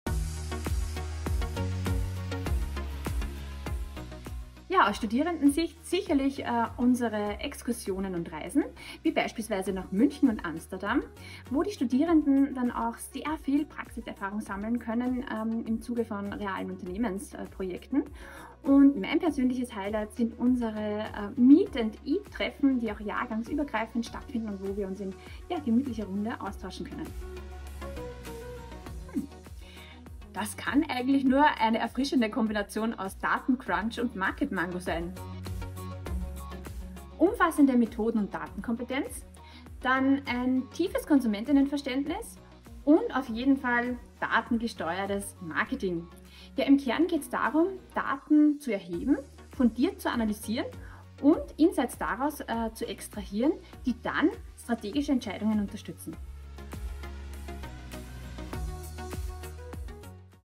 ✨💬 3 Fragen an Consumer Research-Studiengangsleiterin Julia Krall – Teil 2 💬✨
Im zweiten Q&A-Teil mit Julia geht’s unter anderem um die herrlich frische Kombination aus Daten-Crunch und Market-Mango ;) ✨🍦📊🍈

Über die Video-Serie: Während unsere Studis die wohlverdiente Sommerpause genießen, holen wir zur Abwechslung unsere Studiengangsleitungen vor die Kamera. 🤩📽️